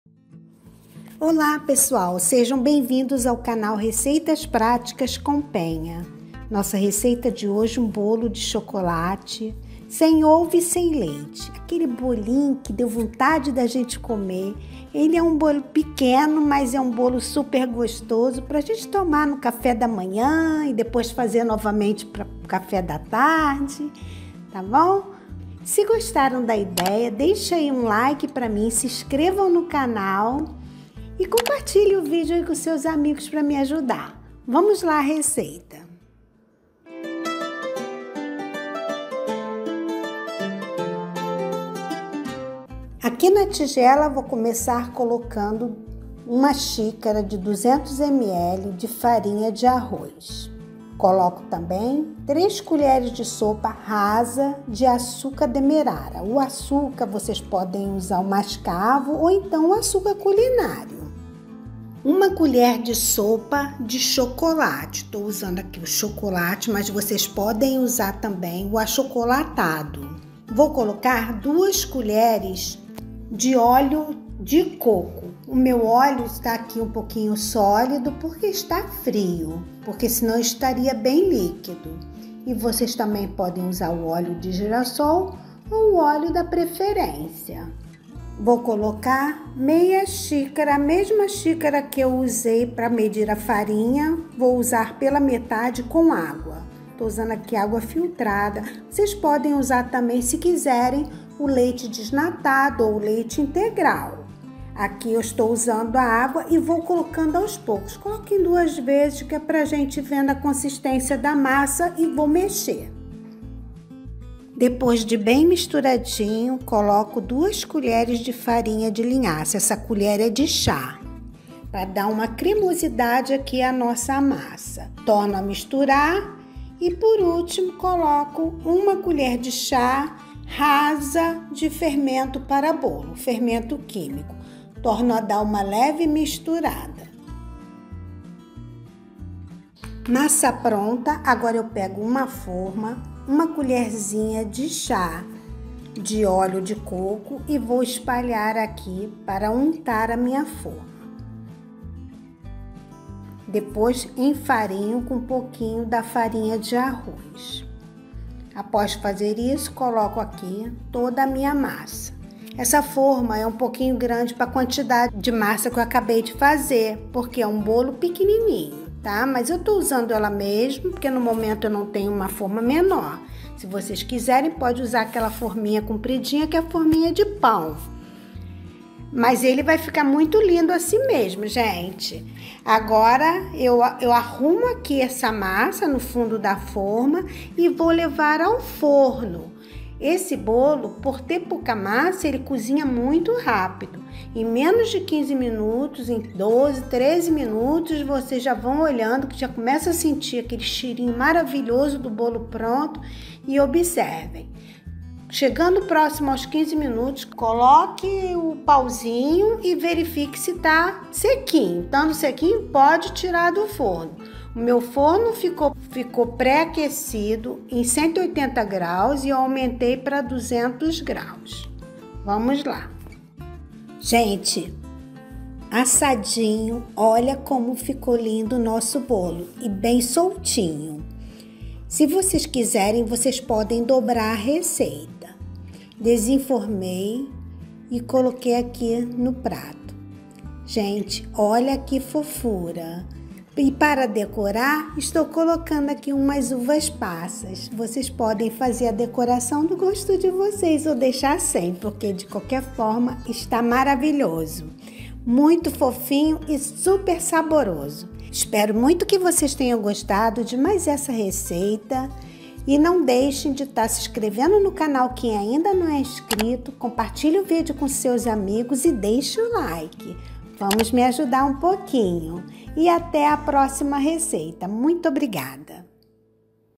BOLO DE CHOCOLATE | SEM OVO, SEM LEITE E SEM TRIGO | Saudável e saboroso!
➧ Ingredientes:
● 1 xícara (de 200ml) de farinha de arroz
● 3 colheres de sopa rasa de açúcar demerara
● 1 colher de sopa de chocolate (ou achocolatado)
● 2 colheres de óleo de coco
● 1/2 xícara de água
● 2 colheres de chá de farinha de linhaça
● 1 colher de chá (rasa) de fermento
● Uvas passas (opcional)

➧ Modo de preparo:
● Siga as instruções no passo a passo do vídeo.